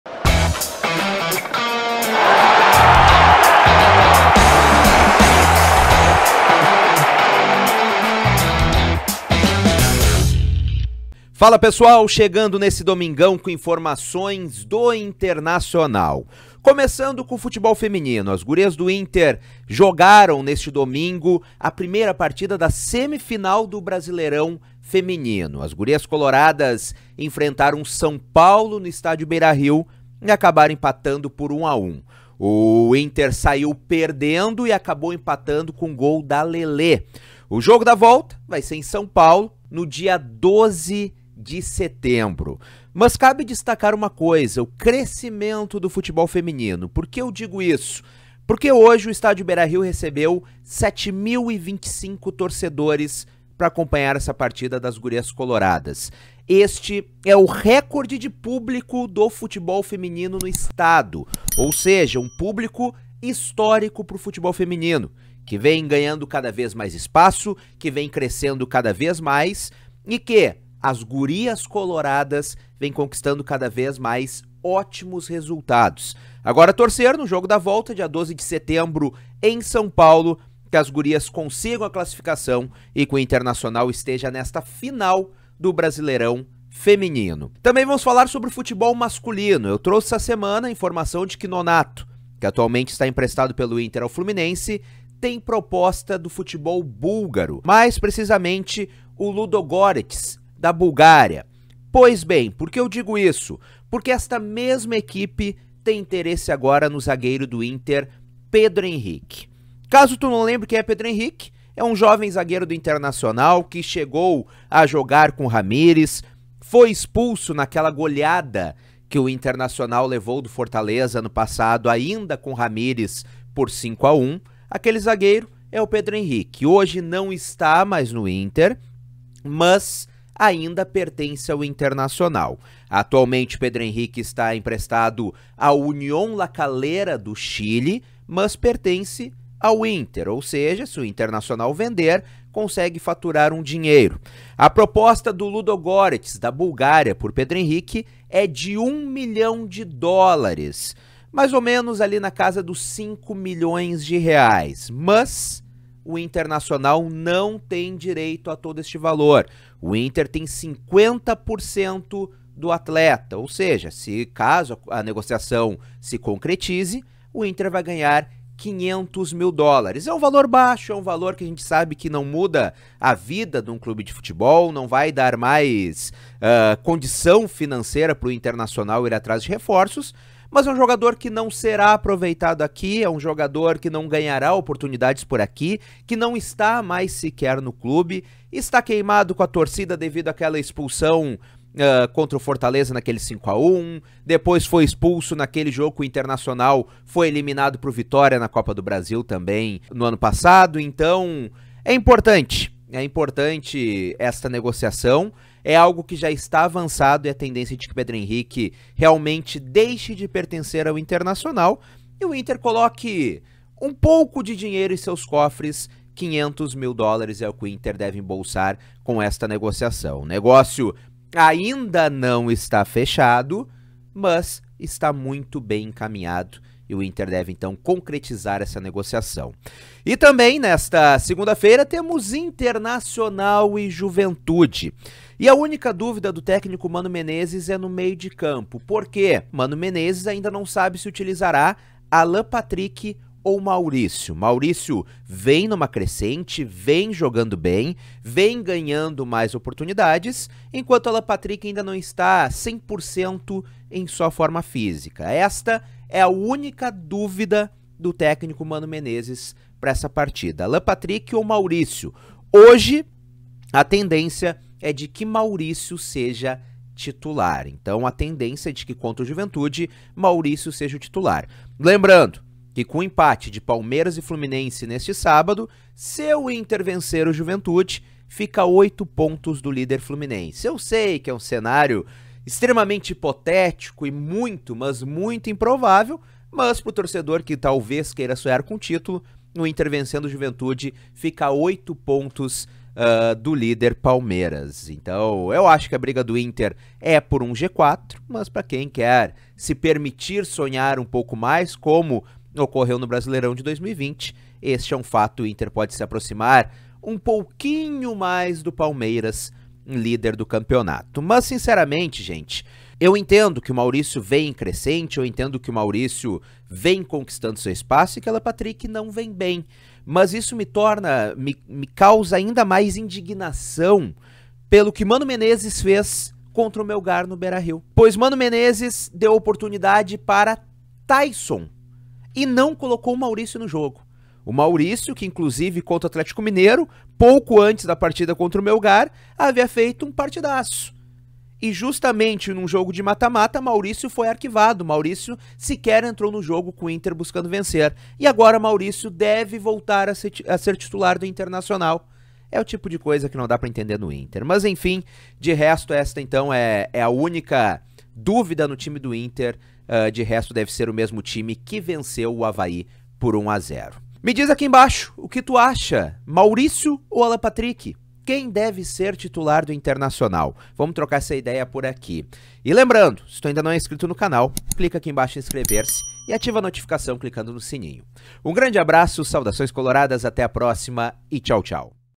INTER RECEBE PROPOSTA DE EUROPEUS POR ZAGUEIRO | A DÚVIDA DE MANO | RECORDE NO FEMININO
Cadastre-se e aposte na KTO!
Cadastrando-se com o código LENNON, tu garante 20% de bônus no teu primeiro depósito!